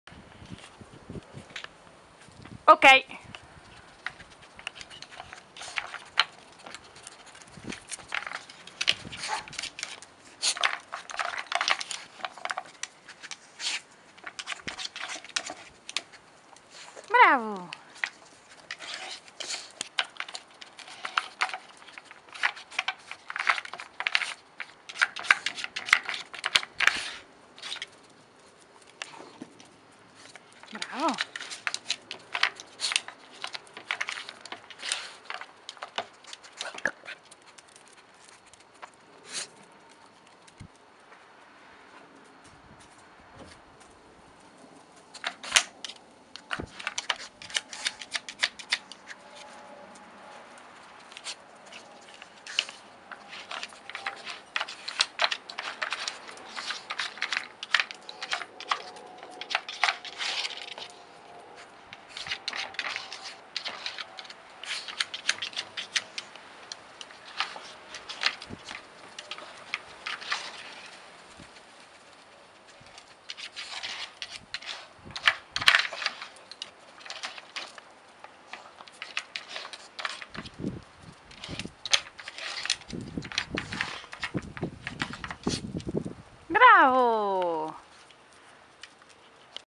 Cairn Terrier Holger - Dog Turbo by Nina Ottosson
Cairn Terrier Holger (Explorer Viking della Costa del Vento) with Dog Turbo, do activity toy by Nina Ottosson